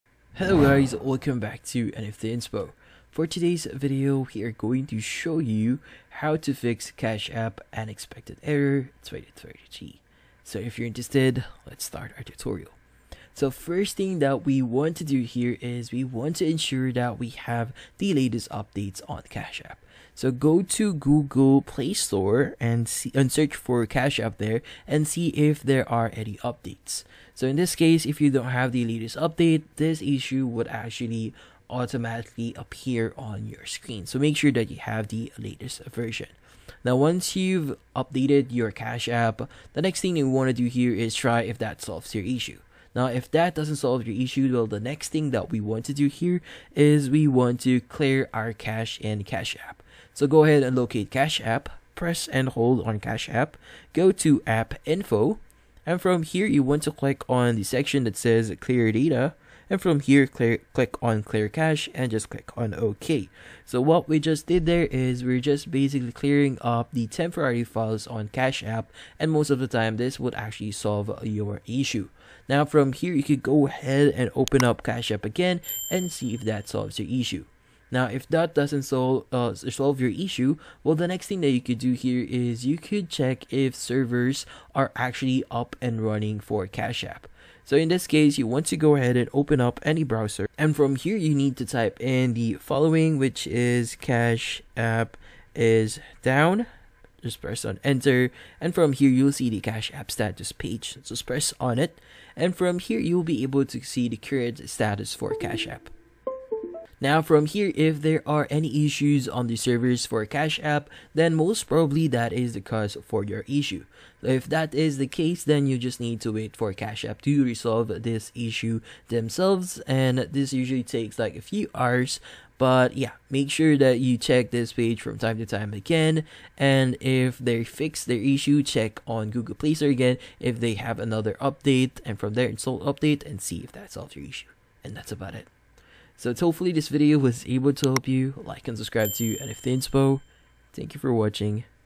How To Fix Cash App Unexpected Error 2025
If you want to be able to resolve unexpected errors on Cash App 2025, then this video will be perfect for you!